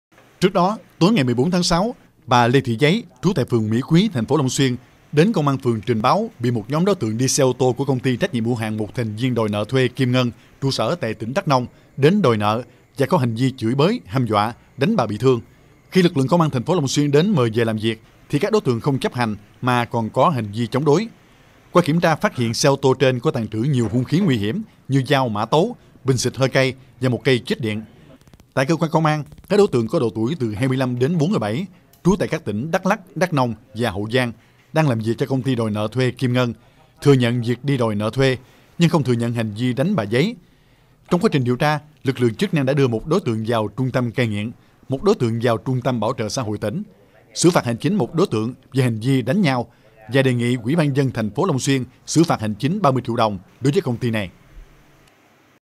XỬ PHẠT GIÁM ĐỐC MANG HUNG KHÍ NÓNG ĐI ĐÒI NỢ THUÊ
Cơ quan Cảnh sát điều tra Công an thành phố Long Xuyên, An Giang cho biết vừa đề nghị UBND thành phố Long Xuyên ra quyết định xử phạt hành chính đối với Giám đốc và nhiều nhân viên Công ty đòi nợ thuê, mỗi người 15 triệu đồng về hành vi "Tàng trữ vũ khí thô sơ, công cụ hỗ trợ trái phép".